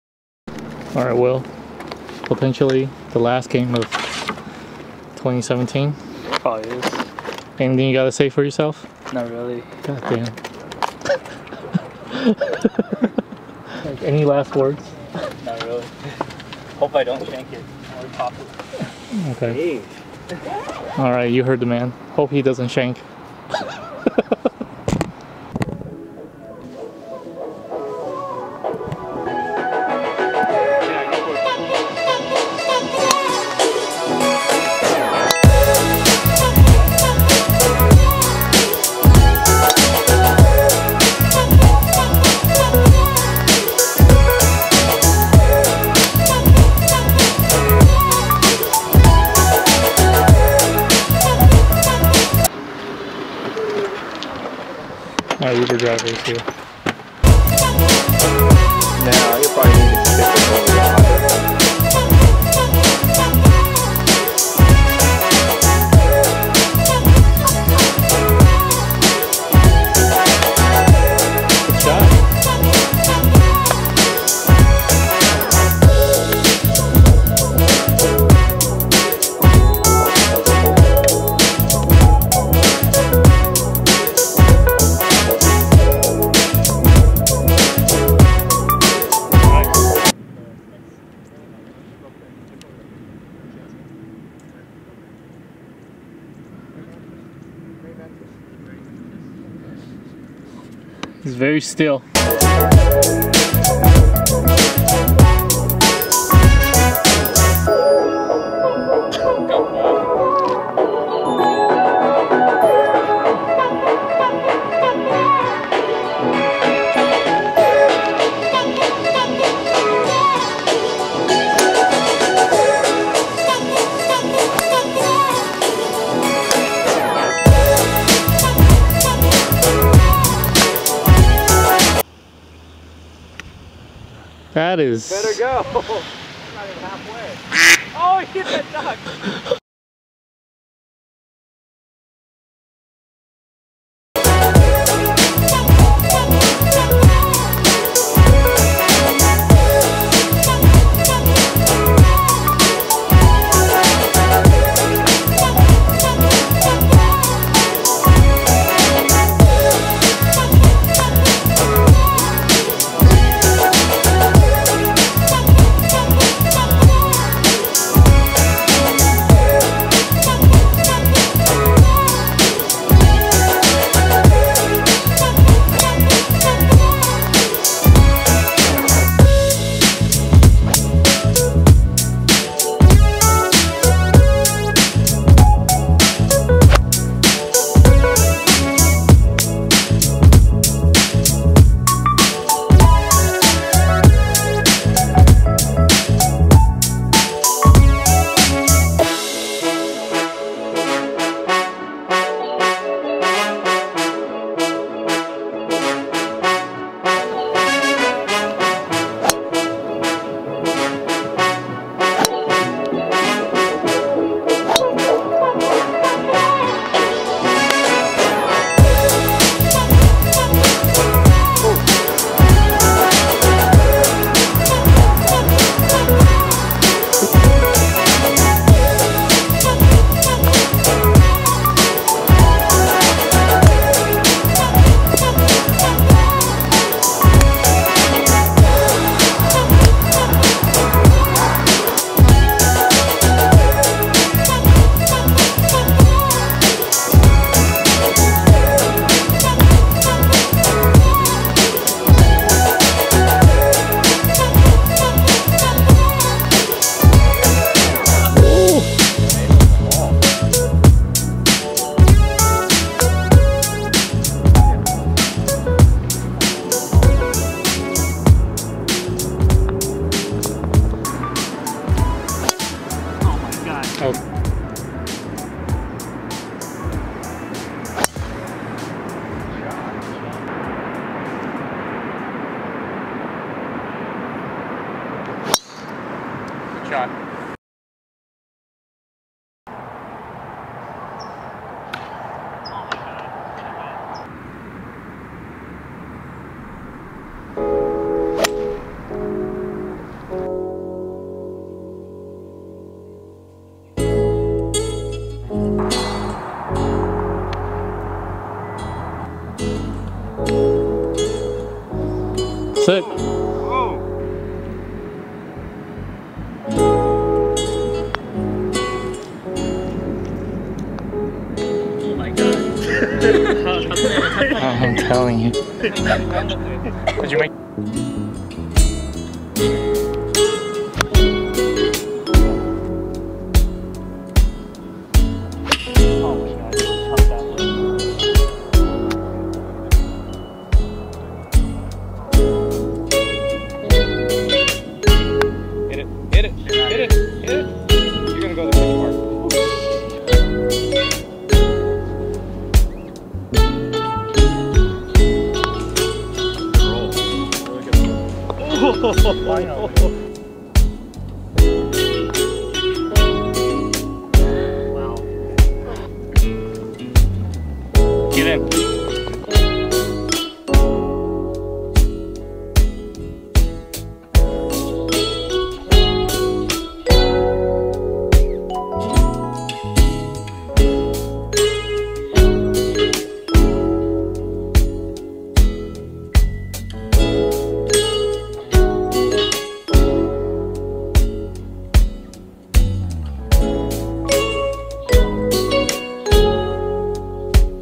December 9, 2017 - Golf Day
The last time in 2017 all 3 of us play golf together.

Looking forward to 2018 - lower scores!